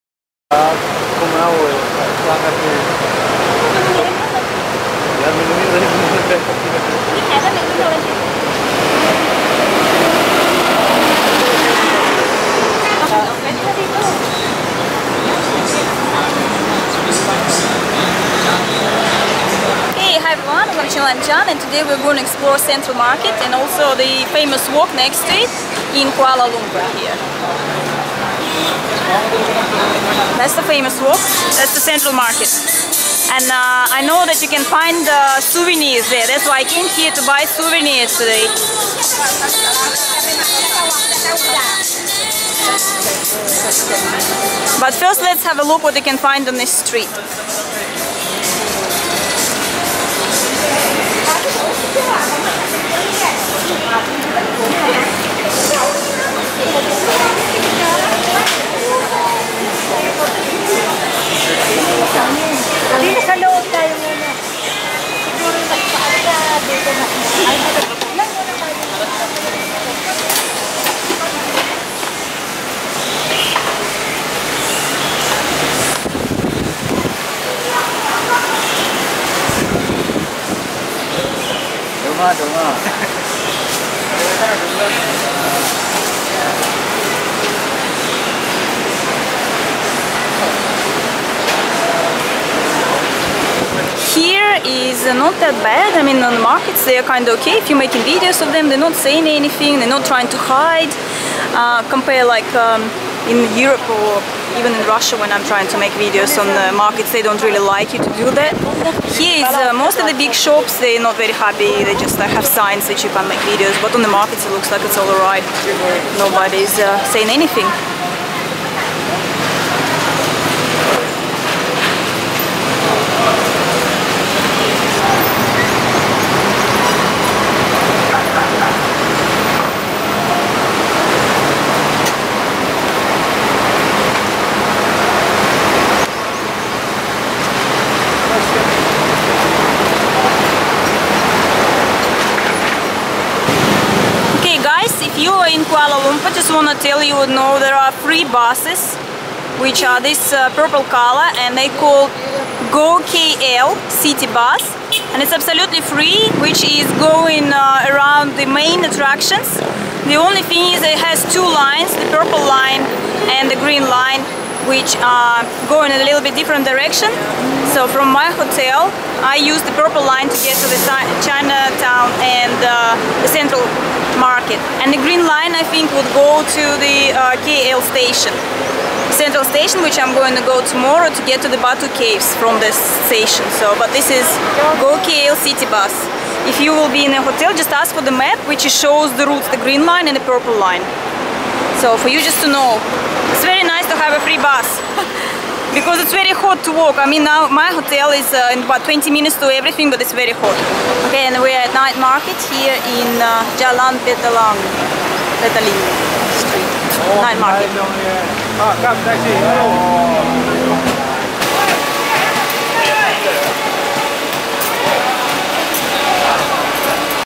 Malaysia Kuala Lumpur, Free Bus, Markets
Russian girl in Kuala Lumpur, Free buses and walking around the markets, Malaysia. Russian girl travels to Asia.
Malaysia, Travel to Malaysia, KualaLumpur, Malaysian, Malaysia Kuala Lumpur